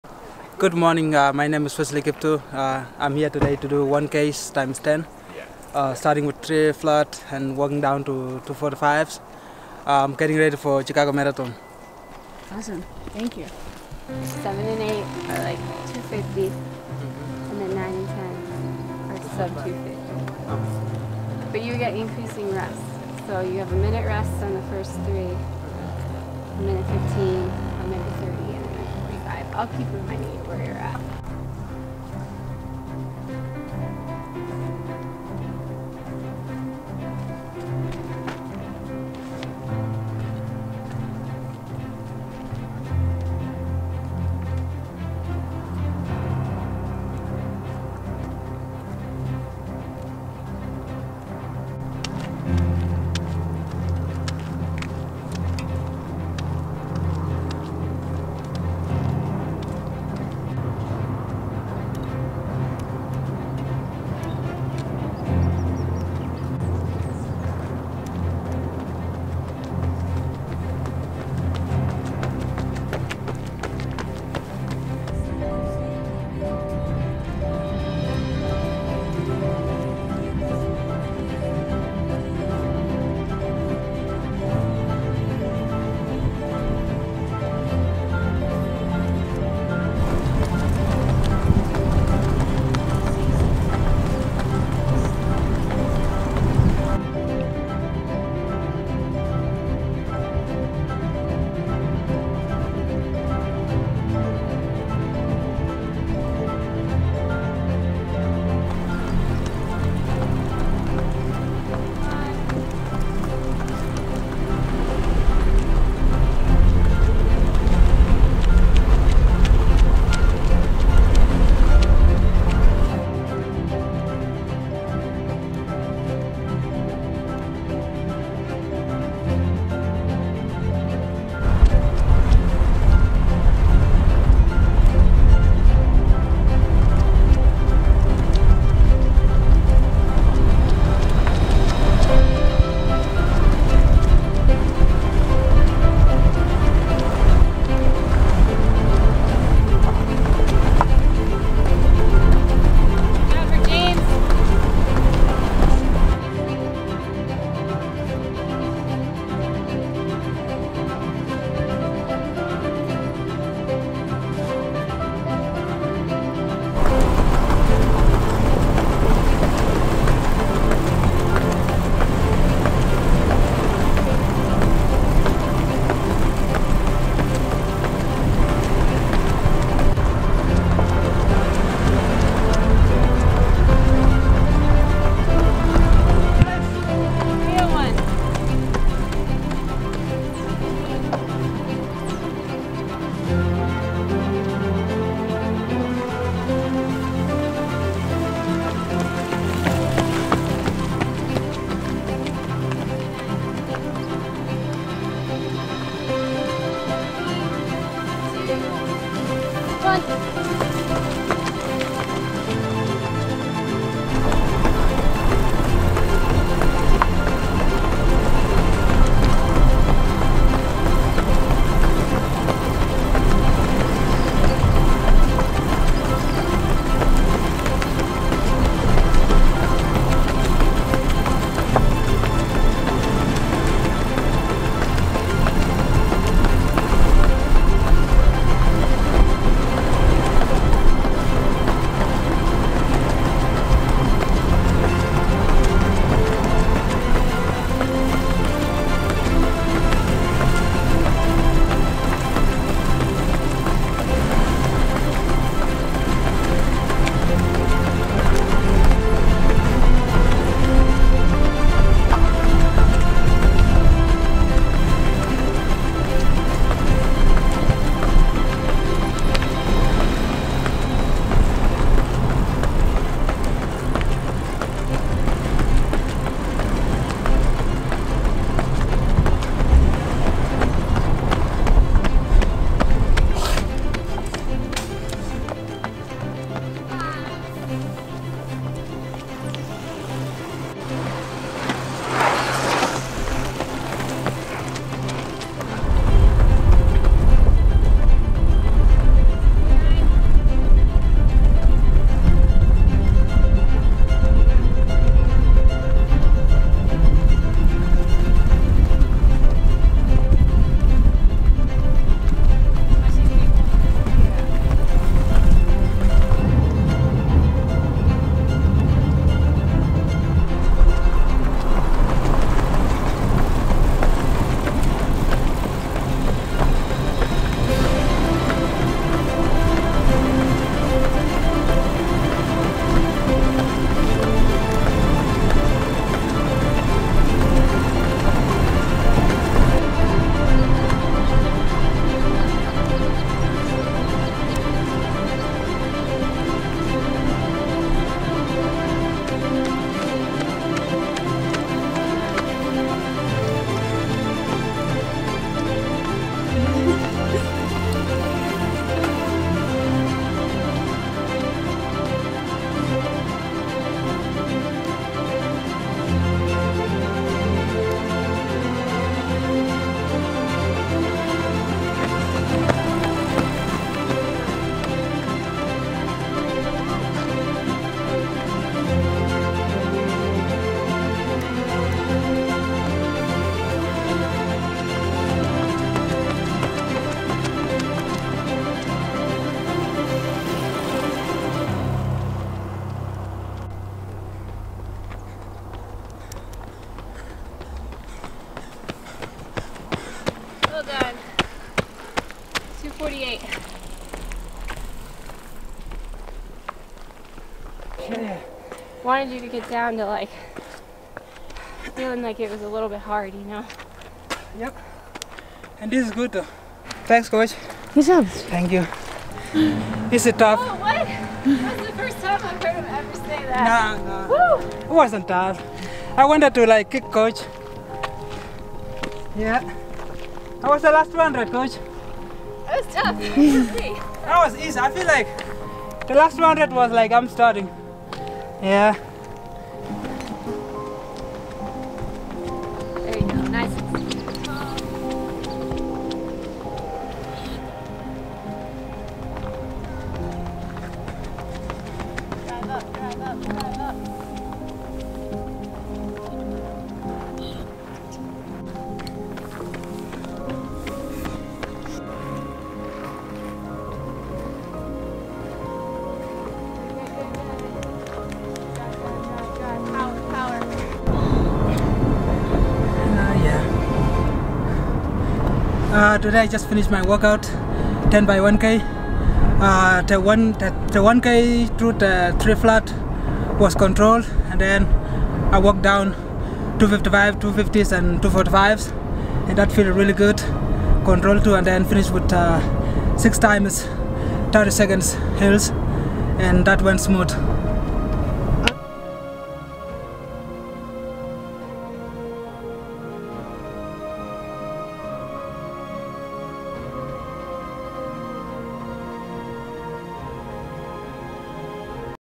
WORKOUT WEDNESDAY w/ Wesley Kiptoo
Paced by Coach Jenna Wrieden on the bike, HOKA NAZ Elite athlete Wesley Kiptoo runs a workout in the new HOKA Mach X on the roads in Flagstaff, Arizona as he prepares for his marathon debut at Chicago in October!

WORKOUT DETAILS: 1k x 10, 6 x 30 second hill sprints

And stay up to date with HOKA NAZ Elite Athletes, Races, & Training